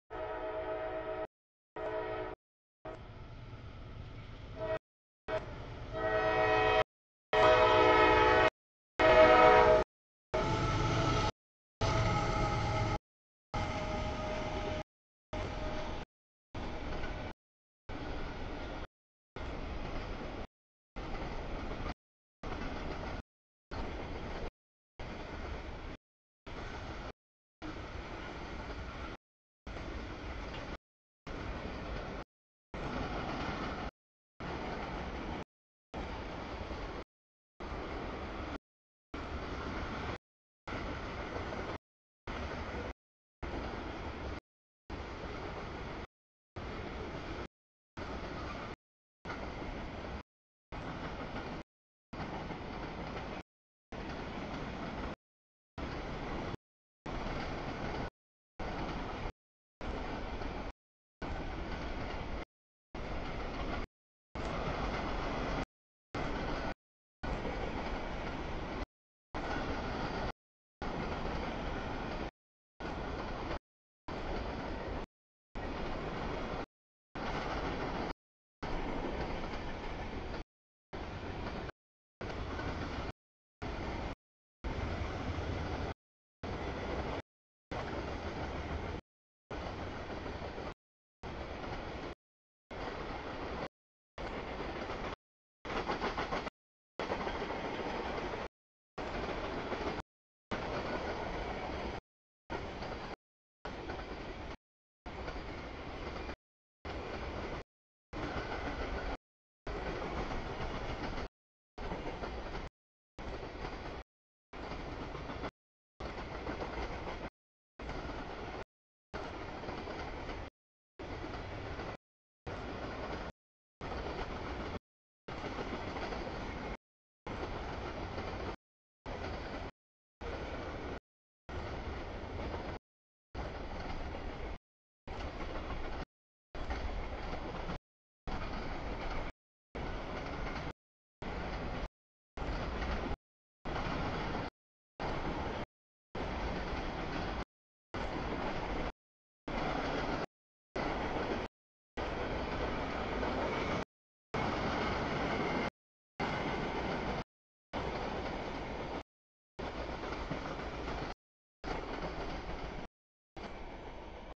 12- 31-2018 10:44 nb mix CSX 146, 3425, 3361 Q190
00:10 sb stack CSX Q004
00:28 nb stack trailers CSX 103 Q009
01:48 sb mix CSX Q403
01:53 nb stack CSX 3279 Q157
03:32 nb mix CSX 3344 Q438
04:13 nb auto rack CSX BNSF Q261
05:01 sb tanks BNSF BNSF K140
05:46 sb stack CSX 4772 Q001
05:55 nb stack CSX Q003
06:34 sb stack trailers CSX Q010
07:17 nb stack CSX 3374, 3148 Q007
07:58 nb empty's CSX 7689, 7840
08:28 nb empty's BNSF 5634, 5506, 5537 K139
09:06 nb auto rack CSX 406 Q234
10:44 nb mix CSX 146, 3425, 3361 Q190
11:15 nb empty's BNSF 7546, CSX 42 BNSF 7375 K141
12:23 sb stack CSX 3060, 381, 3245 Q008
14:27 sb green/blue trash only CSX 52 Q434
14:34 nb mix, epic trash NS 8430 UP 7162 X433
15:23 sb mix CSX 945, 5484 Q439
16:36 sb mix CSX 5499, 7777 Q403
17:18 nb nysw cut, mix CSX 5217, 570, 234 Q410